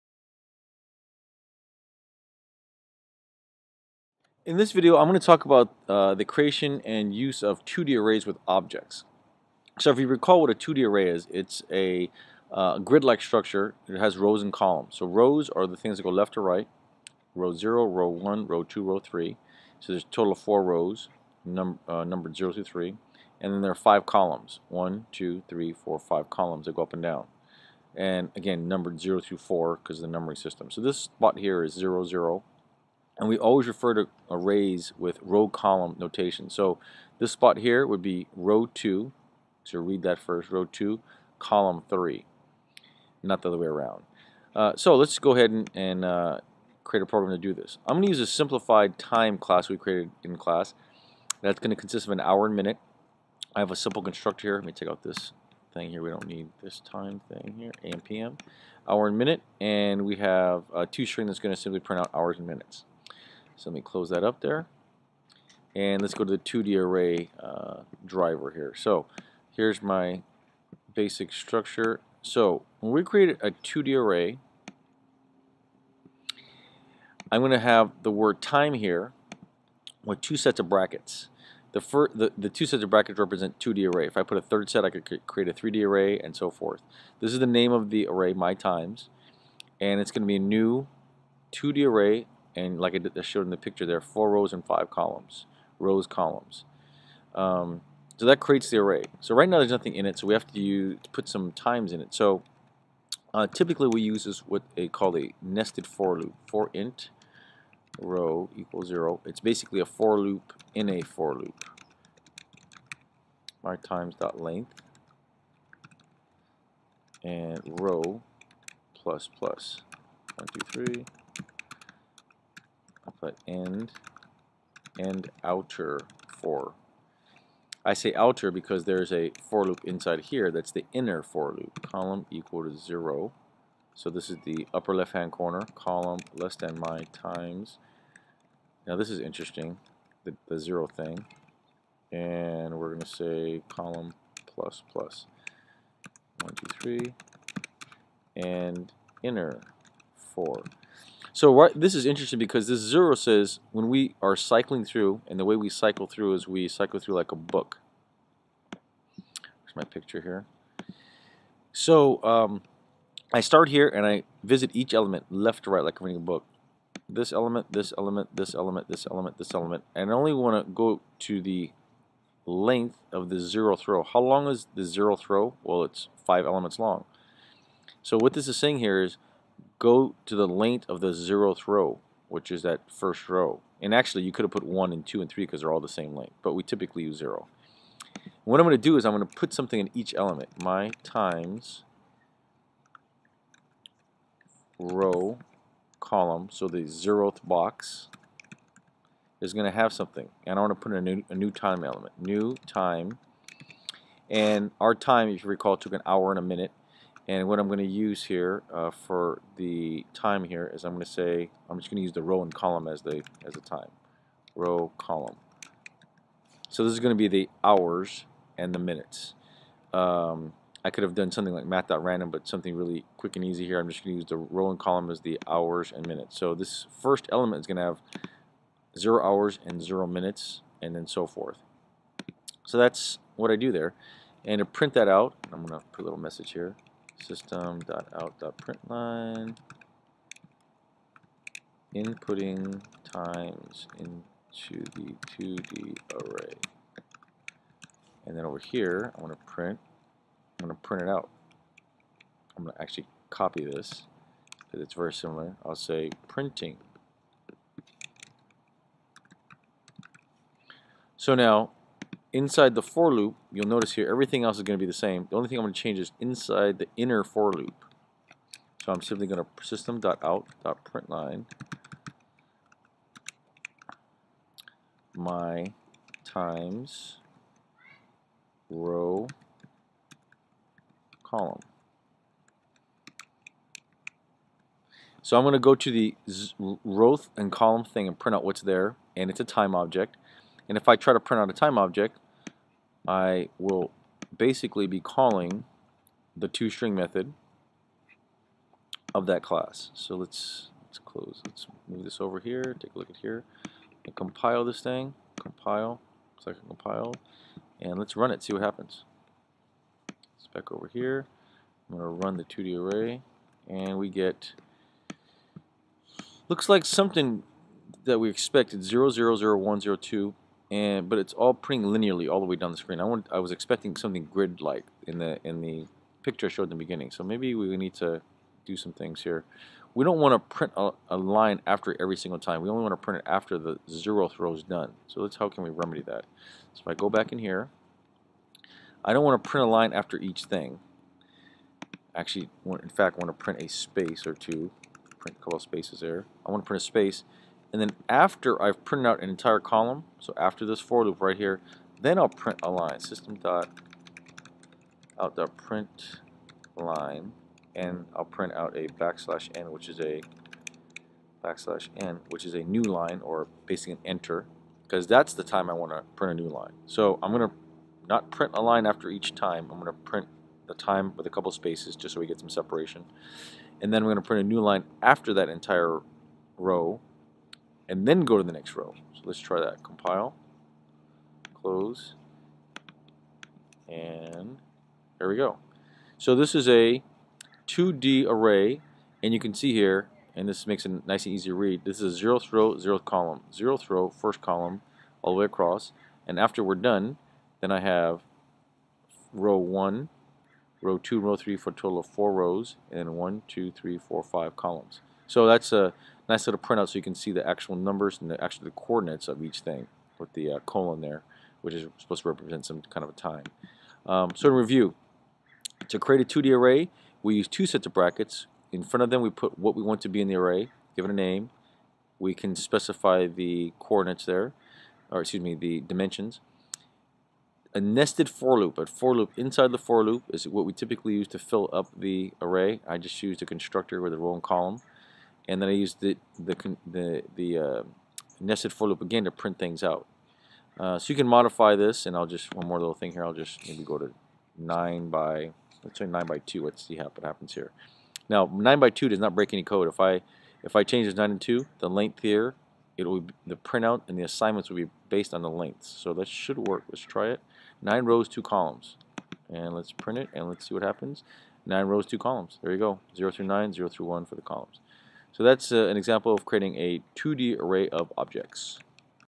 APCS Java 2D Array of Objects
How to create a 2D array that contains objects.